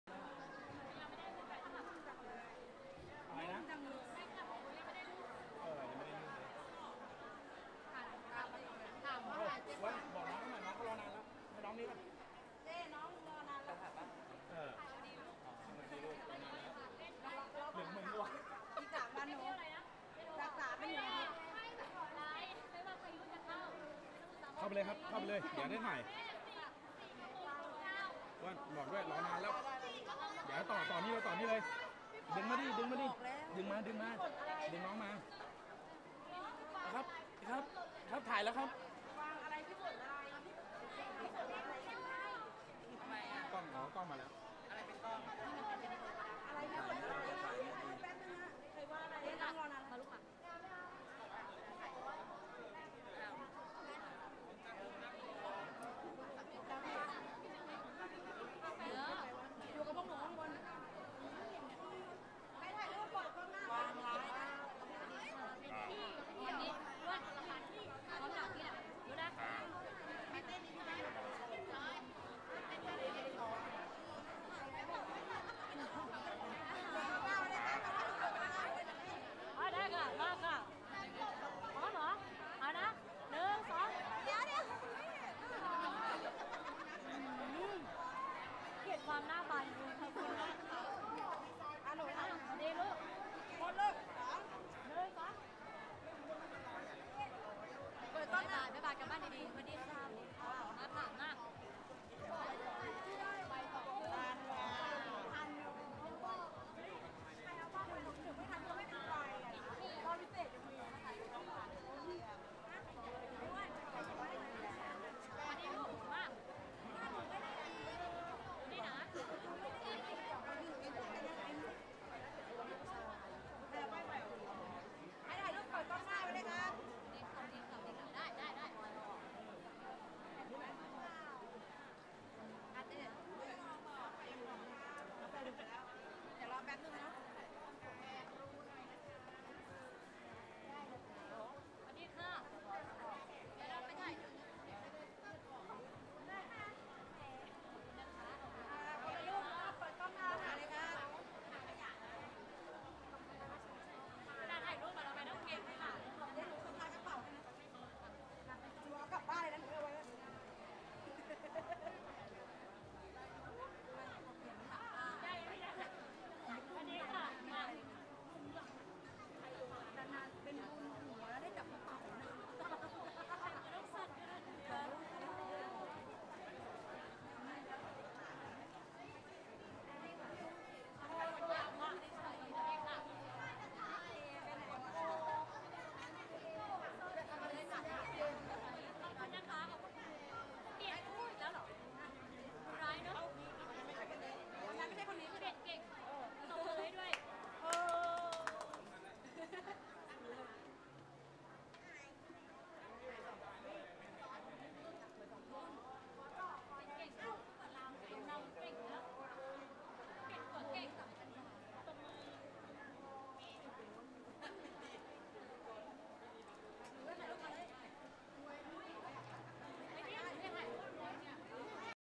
【PhetchaburiDance Workout for weight loss】รำวงเพชรบุรี✿RODE VideoMicro♡รักนะจ๊ะ♡
name of Dancer team : Nongbonapaporn,Phetsopa
name of Music Band : MangKoodPhet (mangM khootH Phet)
Show location : Ban Donklang,Photharam,Ratchaburi,Thailand
Date : August18,2018♡★